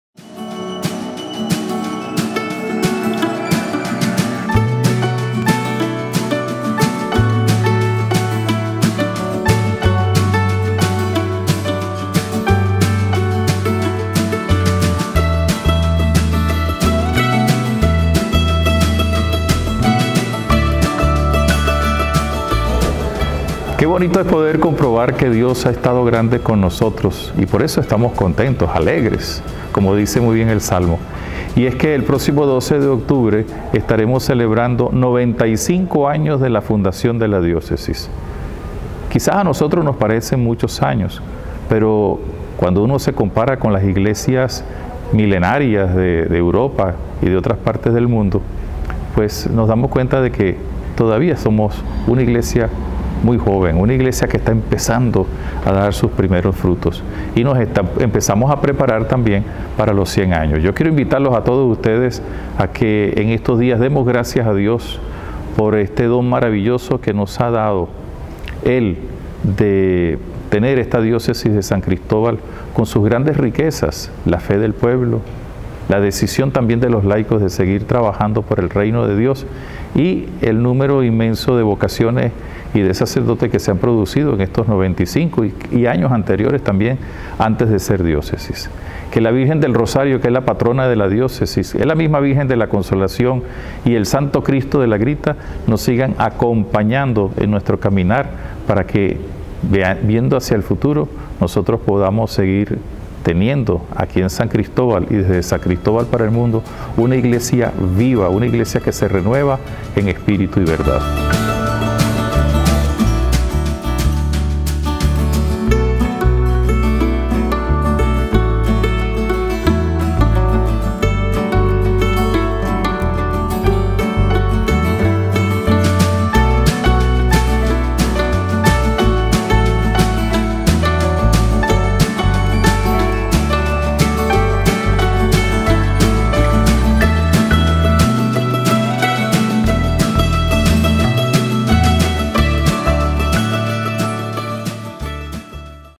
95 AÑOS DE LA DIÓCESIS DE SAN CRISTÓBAL || Mons. Mario Moronta
-XXVIII va Edición del VideoBlog del Obispo del Táchira, +Monseñor Mario Moronta.

-TEMA: "95 AÑOS DE LA DIÓCESIS DE SAN CRISTÓBAL".

-Una Producción del Diario Católico
*La Roca Producciones - 2017*
Parroquia Santa Bárbara de Rubio - Táchira - Venezuela